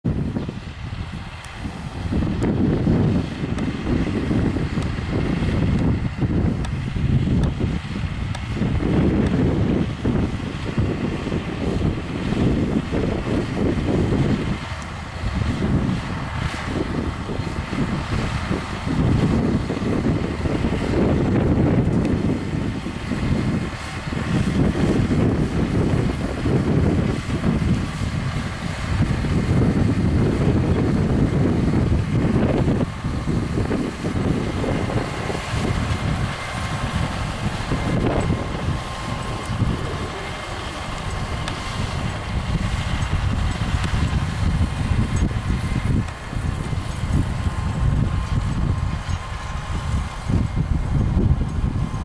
Westbury Ploughing Part 2
Also ploughing in the same paddock near Westbury, in Northern Tasmania this afternoon (6th October 2021) was a John Deere 6150R with a 4 furrow Kverneland reversible mouldboard plough.